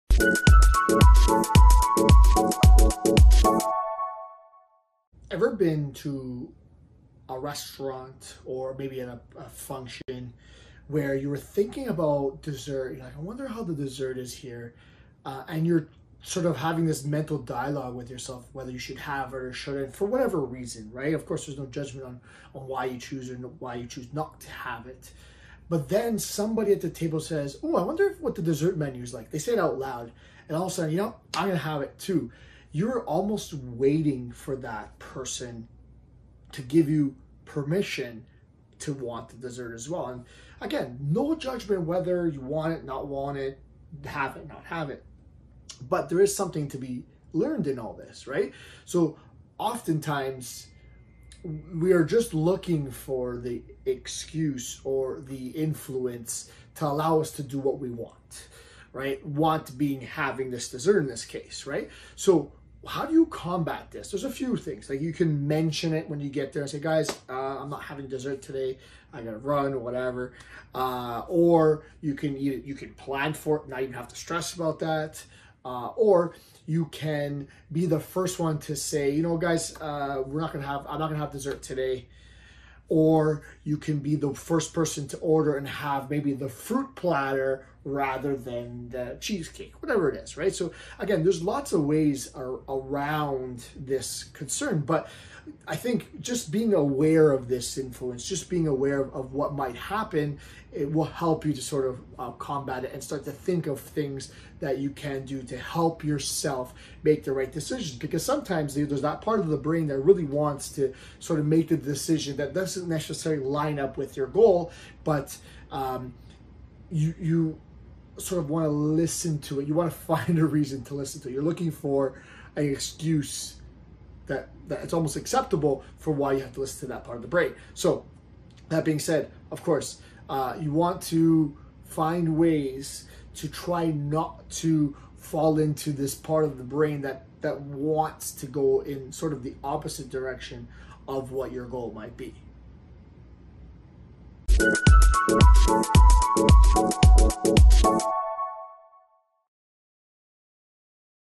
Looking for Permission
Often we are looking for the excuse or permission to do what we want or eat what we want, and it's important to be aware of this and find ways to make the right decisions in order to stay on track.